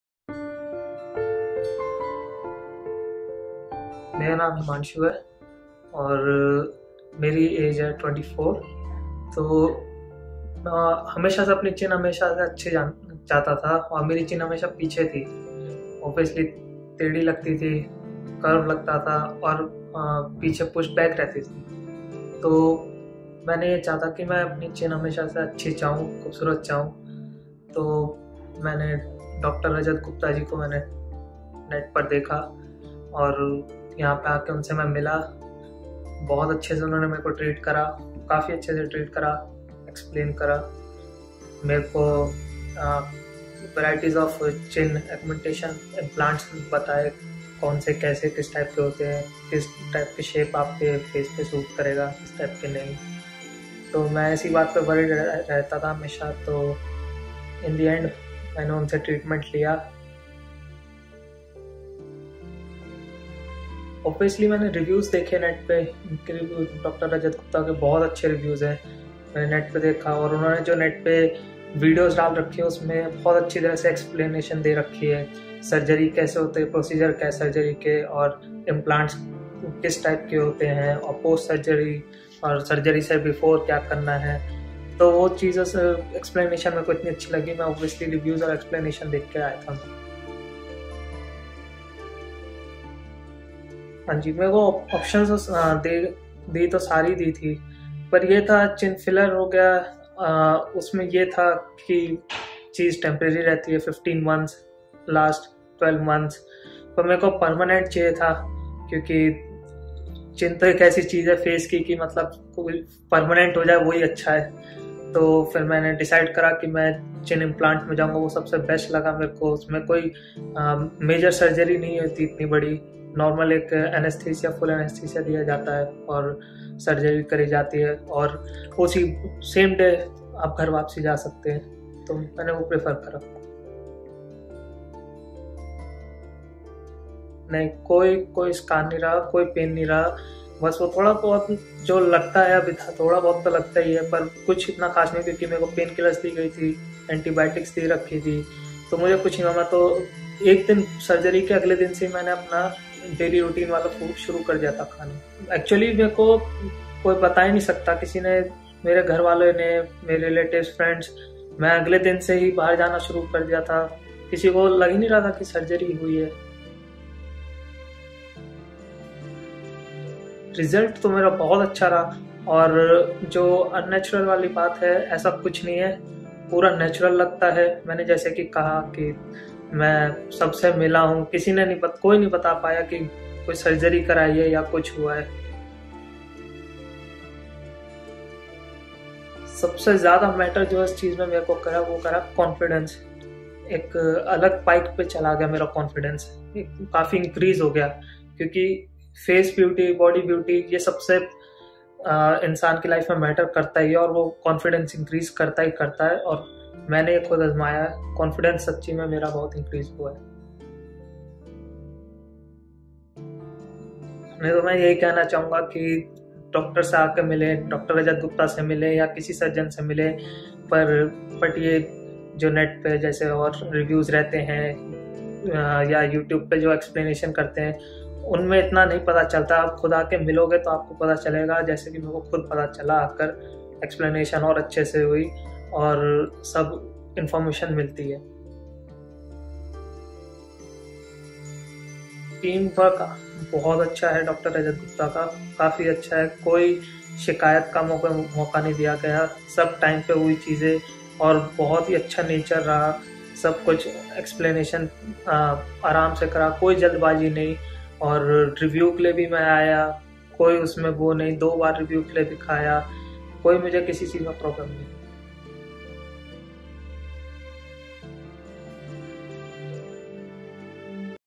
"Happy with the results" | Chin Augmentation Surgery | Patient Testimonial | Dr Rajat Gupta
Mr. Himanshu consulted Dr. Rajat Gupta For Chin Augmentation Surgery. He was not happy with the way his chin was located. Therefore he was searching for the best plastic surgeon and he came across Dr. Rajat Gupta.

He checked online reviews and Videos by Dr. Rajat Gupta and that's when he decided to undergo treatment under his guidance. He booked an appointment with Dr. Rajat Gupta. He had lots of queries regarding Chin Augmentation Surgery but Dr. Gupta explained to him the procedure in detail and cleared all his doubts.

He was continuously looked upon and proper care was taken after surgery. His recovery was pretty smooth and quick. Immediately after surgery, he could do his routine activity. The results were amazing and aesthetically natural.

Watch this video to understand his transformation journey.